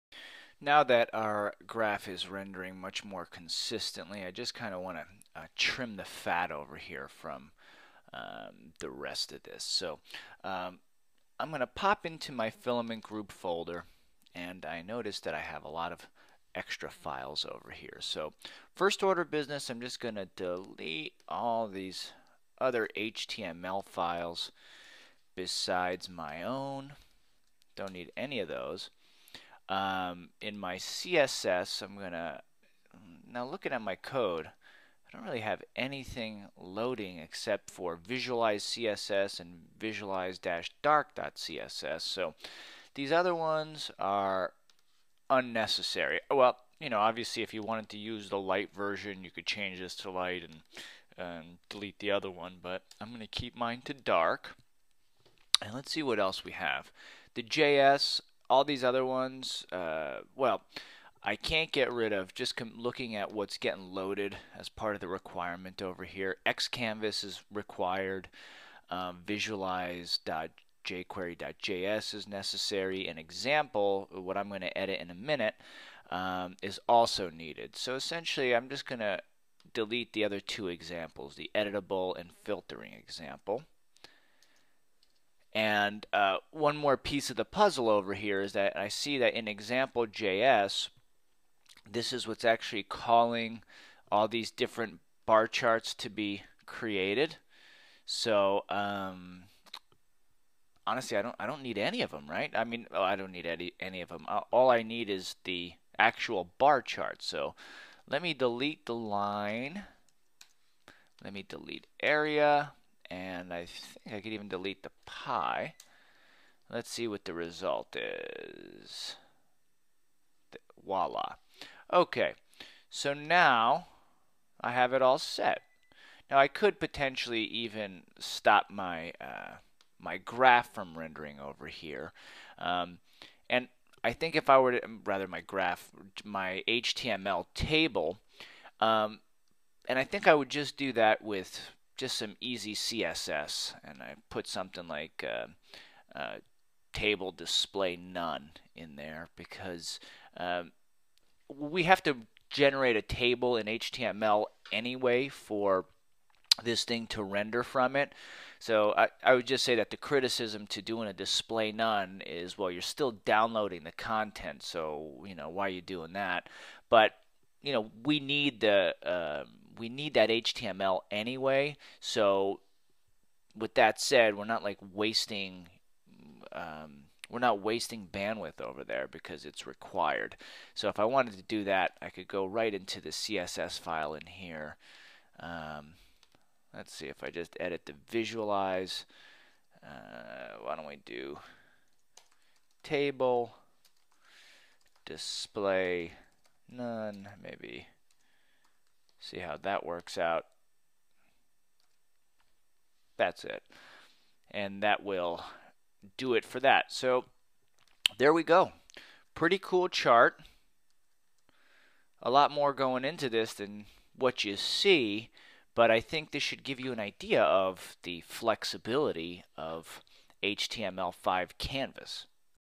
(10/10) Removing Unnecessary Code - Graphing Data with HTML5 Animation
In the final step of our project we'll remove any unnecessary code from our folder to make our implementation as lean as possible.